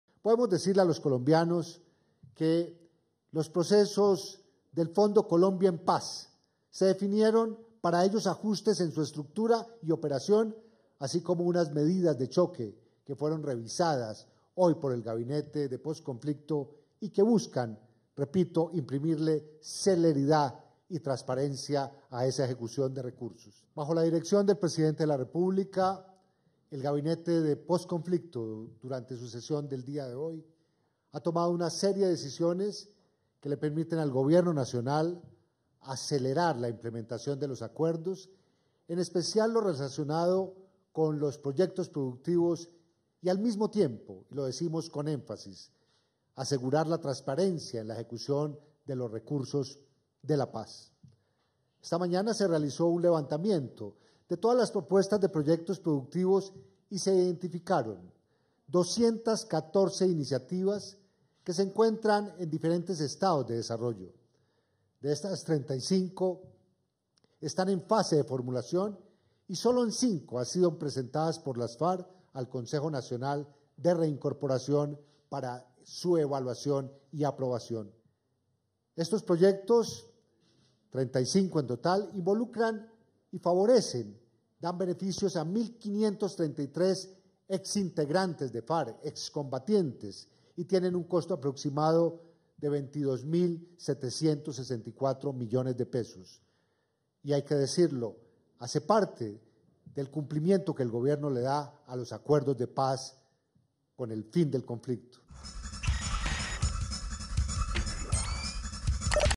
Gobierno anuncia medidas de choque para mejorar transparencia en fondos para la paz
Al término de la reunión del Gabinete del Posconflicto, el vicepresidente de la República, general (r) Óscar Naranjo, anunció la implementación de un nuevo sistema para la ejecución y el desarrollo de los recursos para los proyectos productivos de paz.